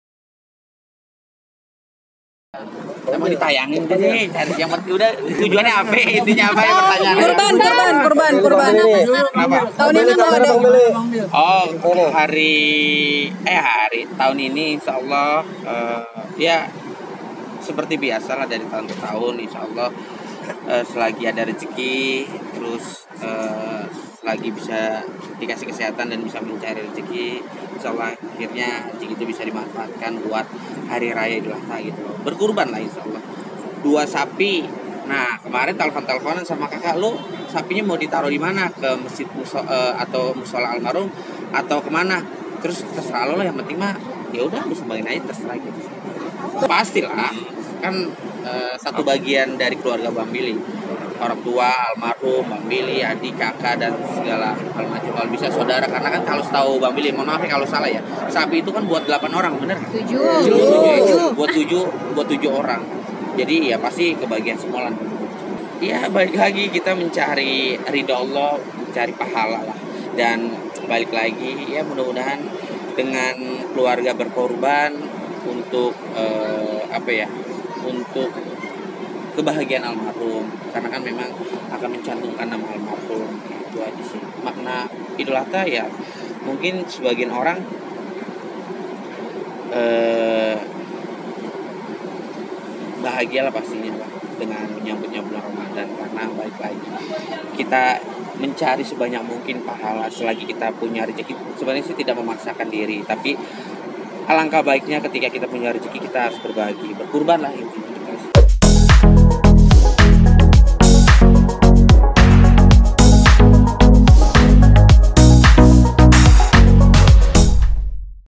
Billy Syahputra Akan Qurbankan 2 Ekor Sapi di Idul Adha Tahun Ini
Grid.ID

EDITORIAL OFFICE

ADVERTISING